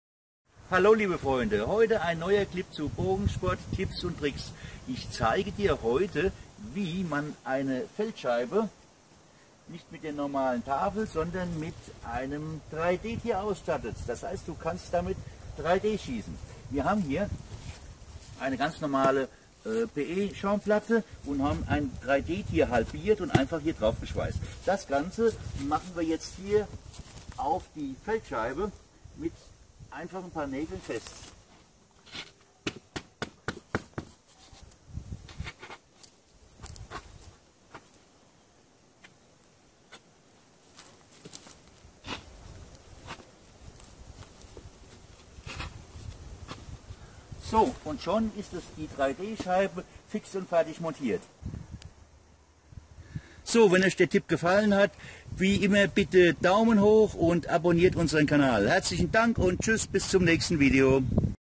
3D Auflage für Bogensport Feld Zielscheibe
So wird ein Feldparcours abwechslungsreicher. Eine Bogensport Feld Zielscheibe kann man schnell und einfach in ein 3D Ziel verwandeln. In Ungarn im Bogenparadies zeigen wir Dir wie Du solche 3D Auflagen einfach selbst bauen kannst. Besuch uns einfach mal